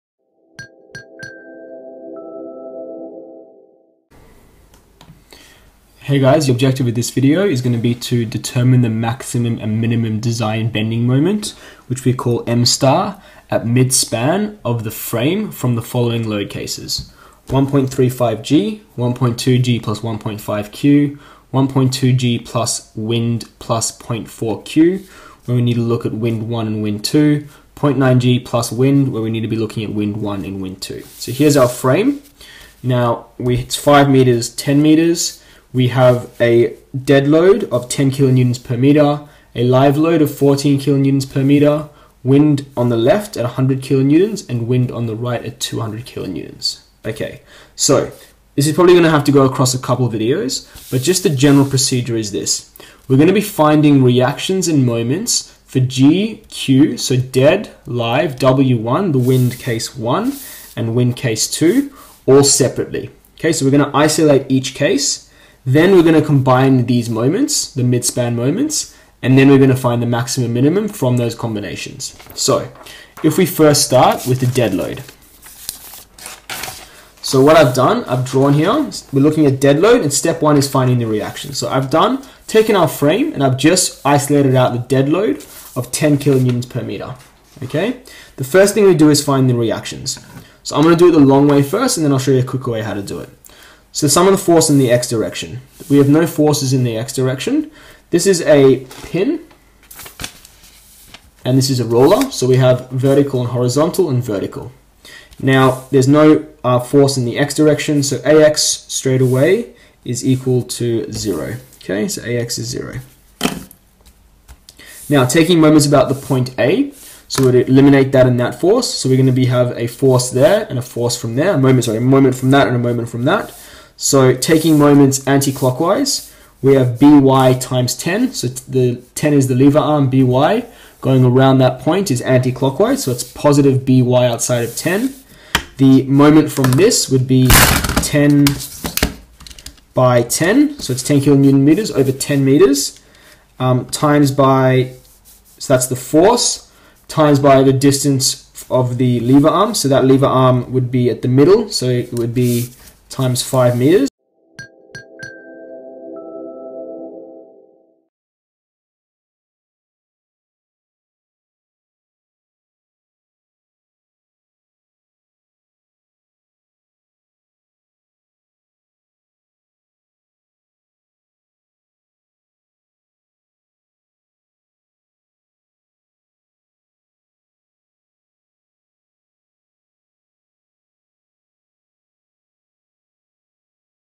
Load Combination Example 1: Dead and Live Load Midspan Moments | Structural Design & Loading
for more FREE video tutorials covering Structural Design & Loading

This video briefly elaborates a comprehensive example of different load combinations that asks to determine maximum & minimum design bending moment at midspan of the frame from the given different set of load combinations on a frame. Very first, the video demonstrates the given load condition graphically & introduces the calculation steps. Next, the video step by step shows how to find out reactions due to the given dead load combinations.

Moving on, the video briefly shows the procedures to compute the midspan moment from different parameters e.g., load combinations, reactions, distance etc. Later, the video shows how to find out max and min design bending moment at midspan of the frame from the earlier computed data. Fundamental on different types of loads and moment calculations is required to understand the core content of this video as well as more practices on different load combinations are required to master the whole thing precisely.